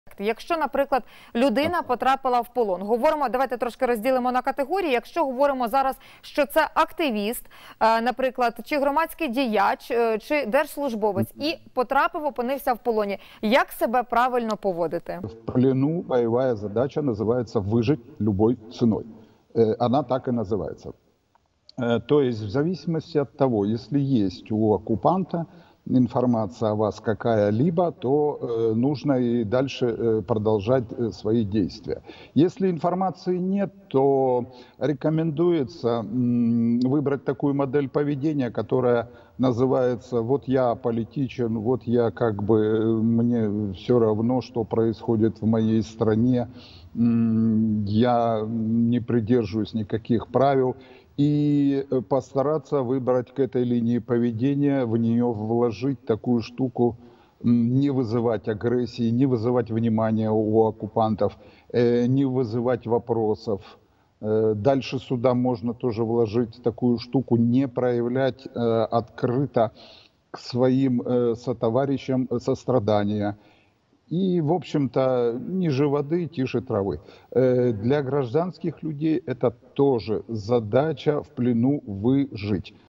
Як поводити себе у полоні – поради психолога | Сьогодні. Головне
Про те як діяти у випадку полону та яких правил варто дотримуватися полоненому говорили у розмовній студії з психологом Олександром Ковальовим.